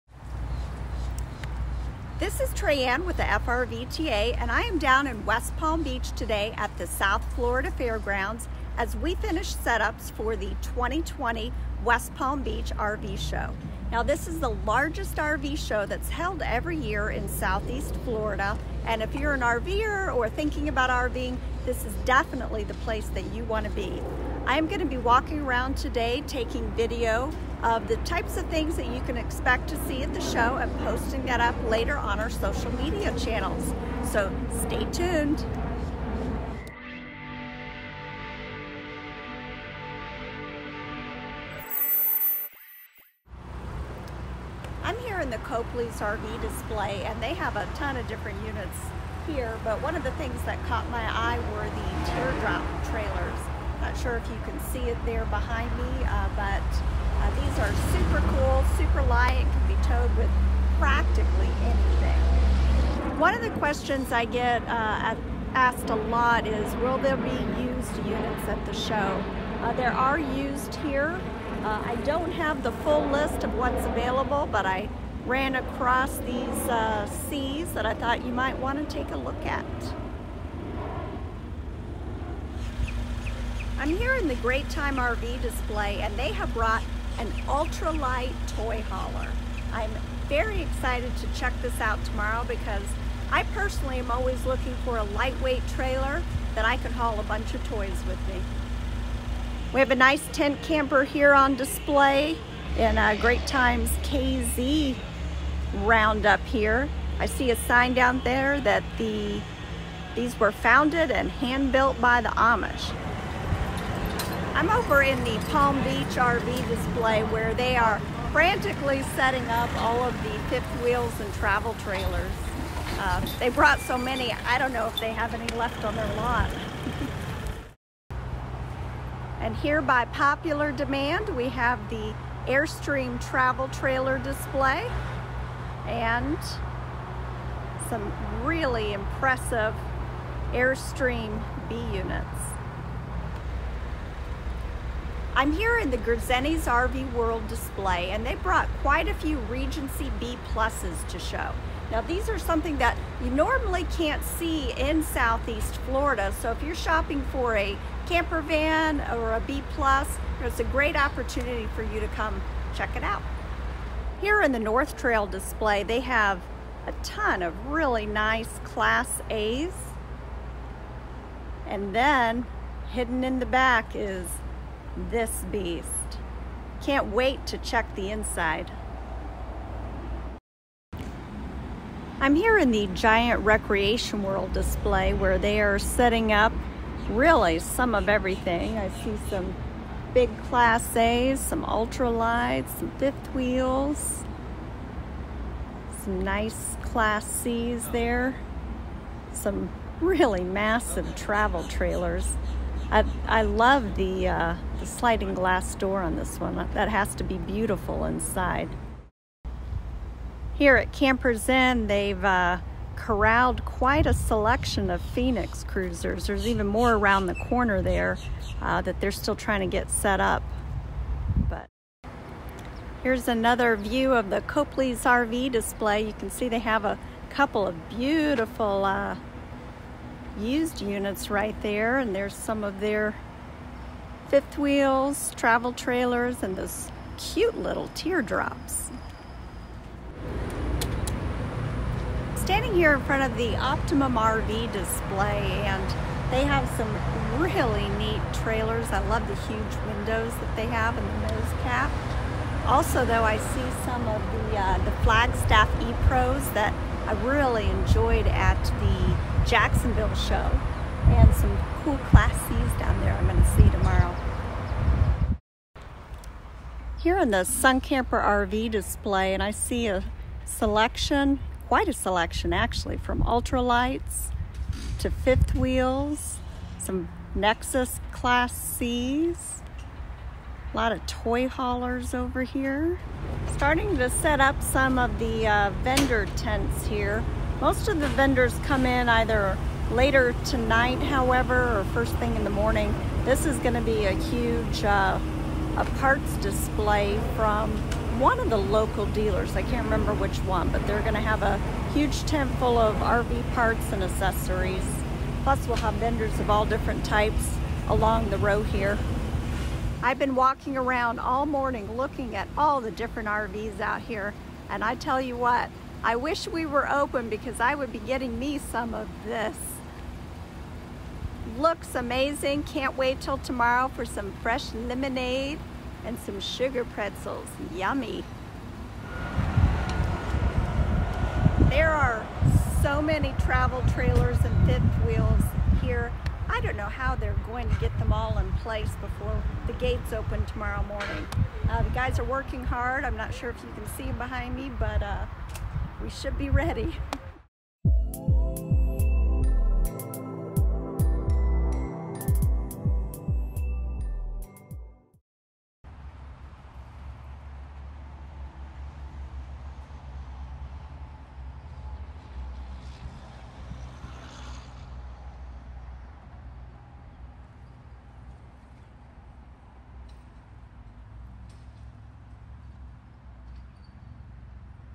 2020 West Palm Beach RV Show Sneak Peek
The gates open at the 2020 West Palm Beach RV Show on Thursday at 10am. Check out our sneak peek of the largest RV Show in Southeast Florida to see why this multi-dealer RV Show is a must-see if you are in West Palm Beach this weekend!